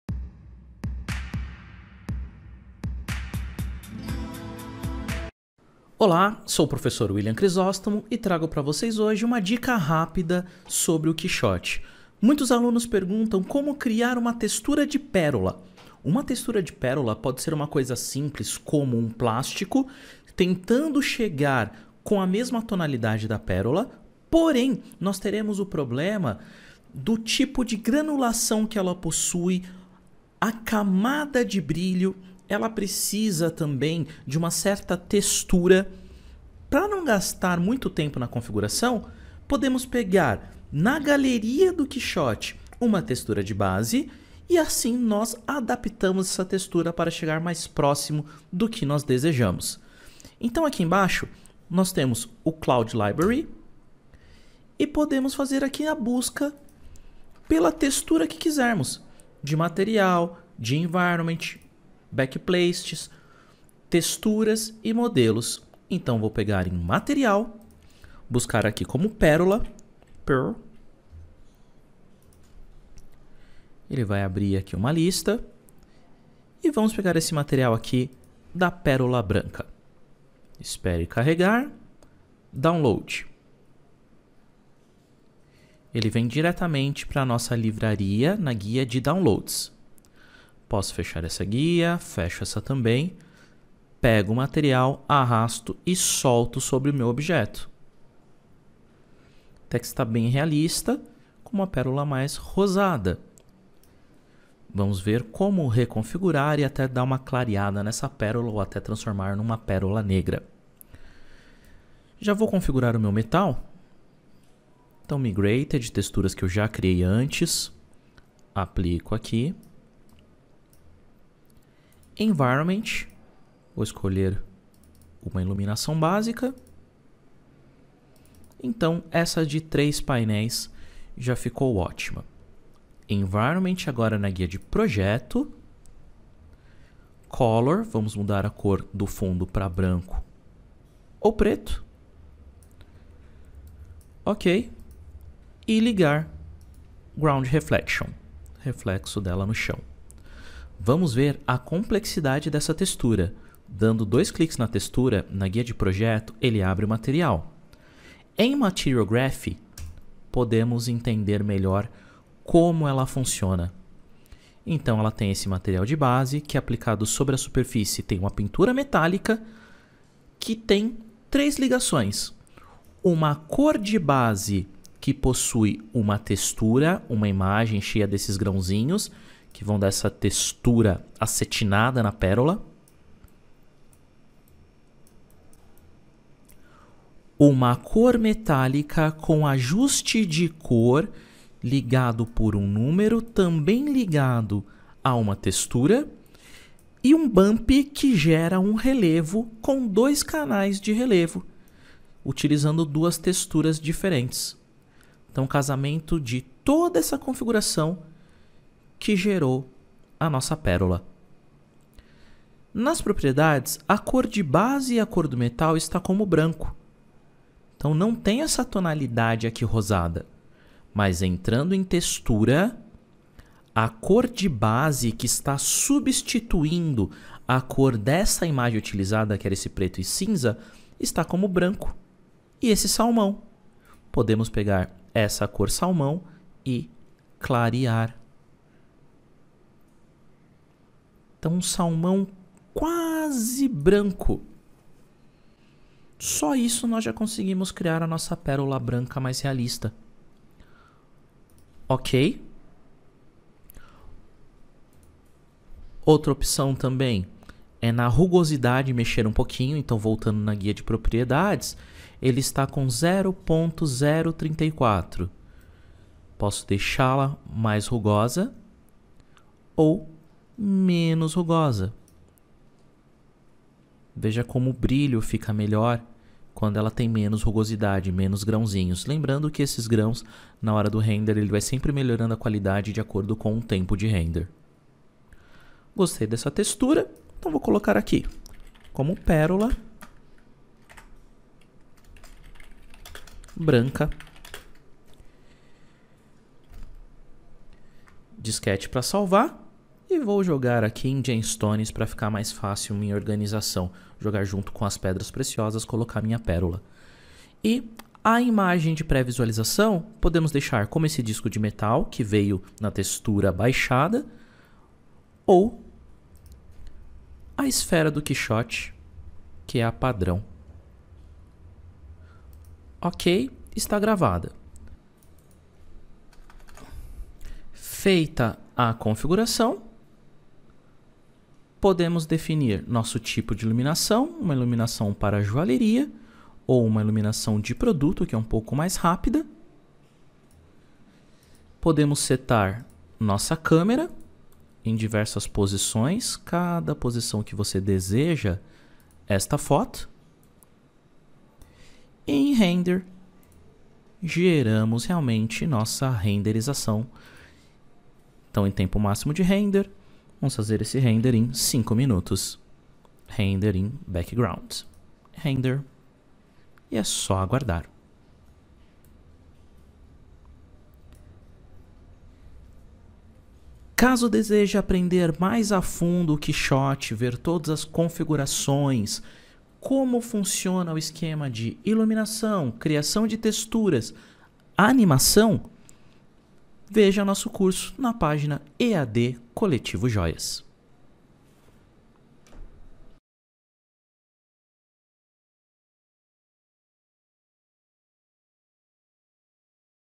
Textura de pérola no keyshot, render para joias
Aprenda a criar uma textura de pérola  com o software Keyshot com um arquivo vindo diretamente do Rhinoceros, Rhinogold e etc.